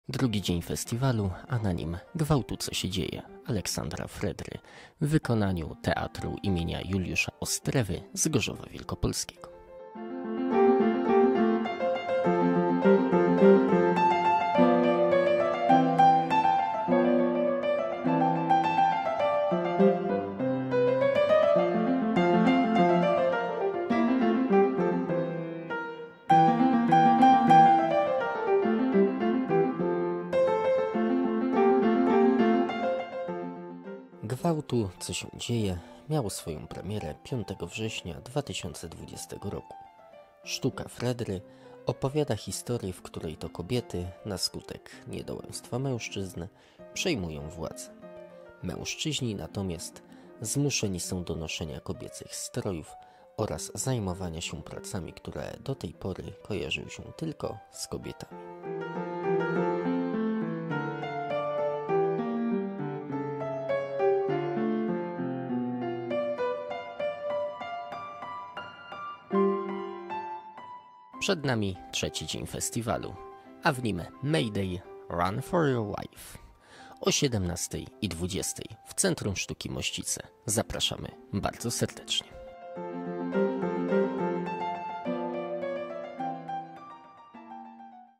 "Talia"- day 2!
Drugiego dnia zaserwowano nam mistrza polskiej komedii - Aleksandra Fredrę i jego "Gwałtu, co się dzieje!" w wykonaniu Teatru im. Juliusza Osterwy w Gorzowie Wielkopolskim.

Relizacja: Nikola Fil, Michał Liguz